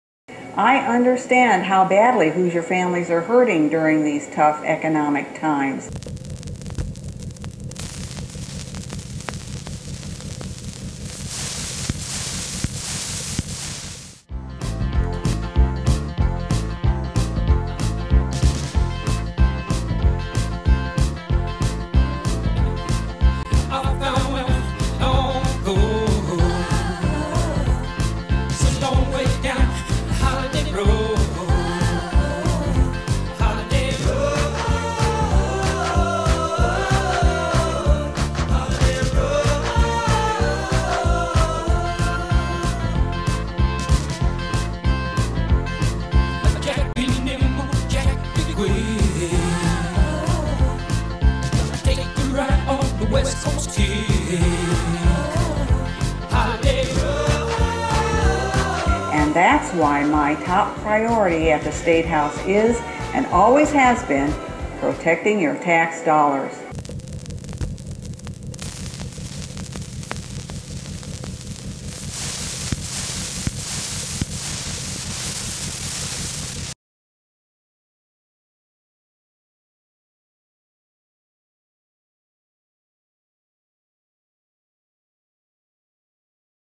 Senator Landske Likes To Travel
Can we trust Senator Landske to protect our tax dollars?